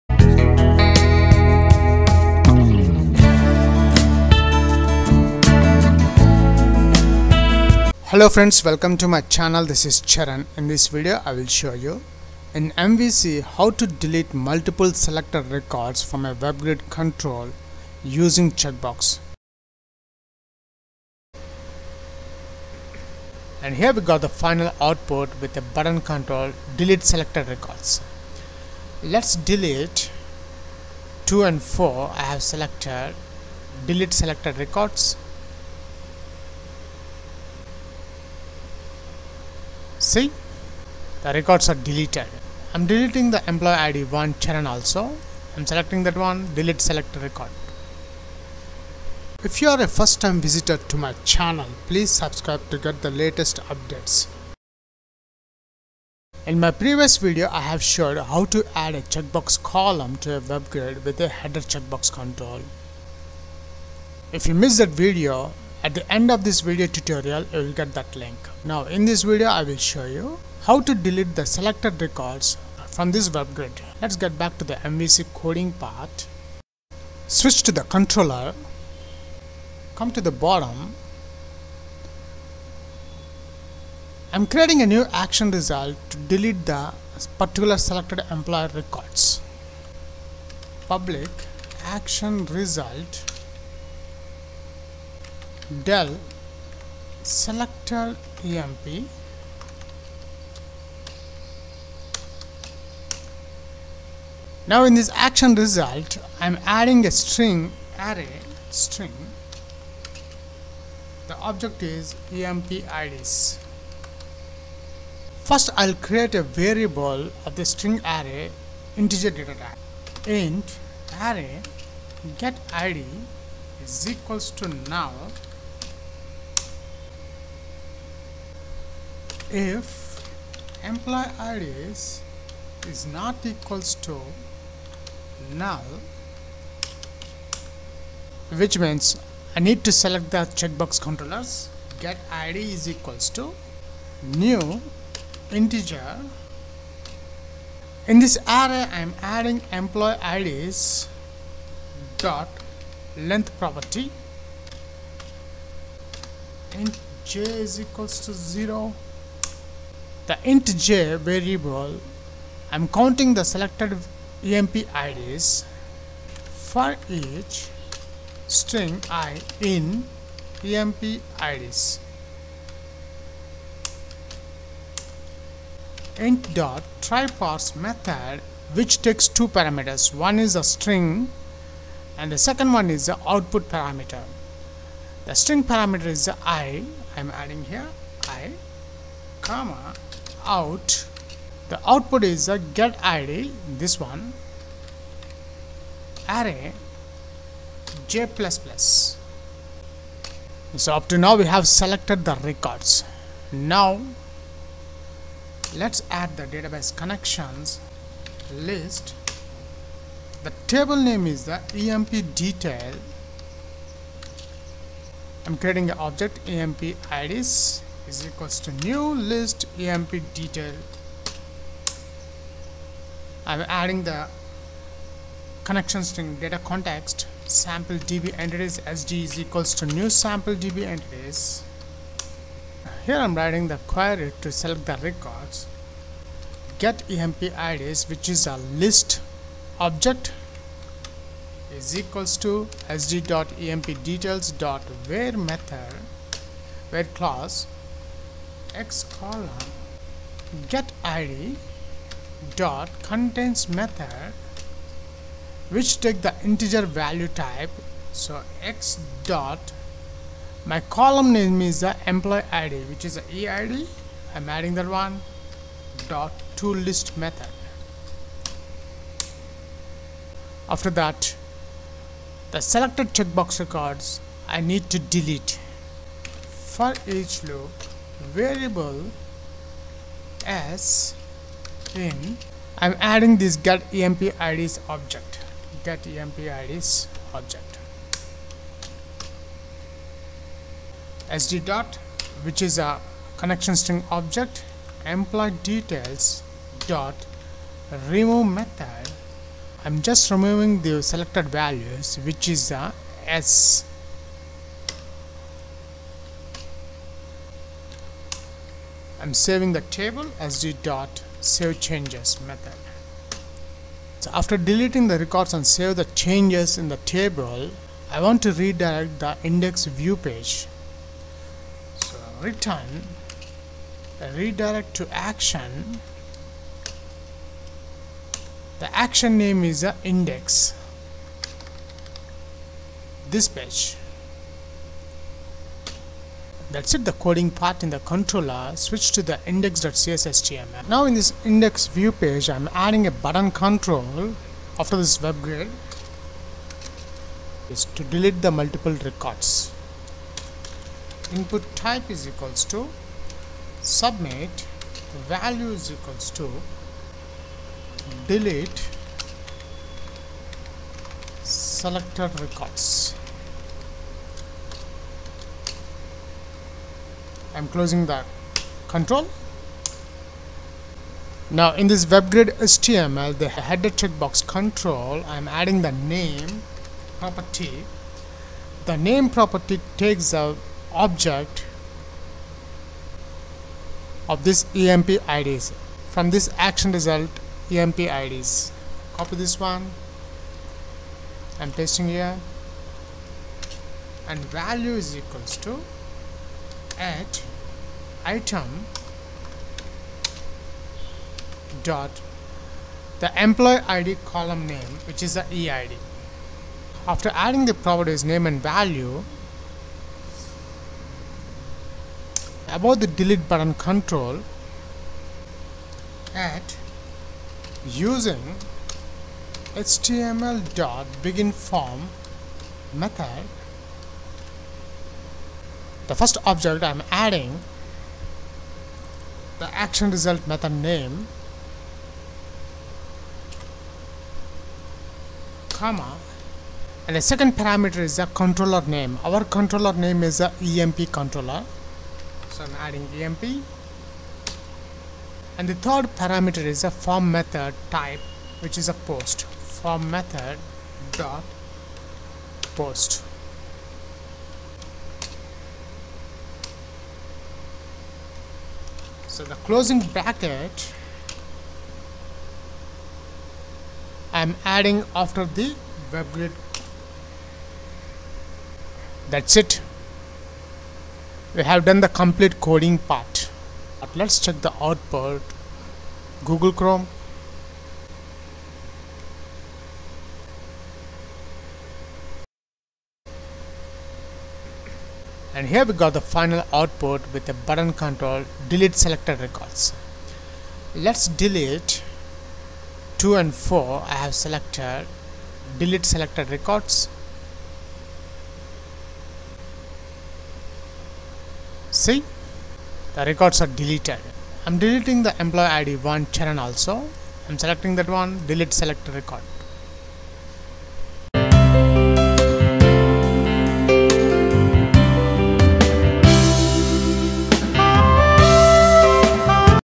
mvc delete multiple rows checkbox example with webgrid
mvc with checkbox delete mulitple selected records complete tutorial with example using c#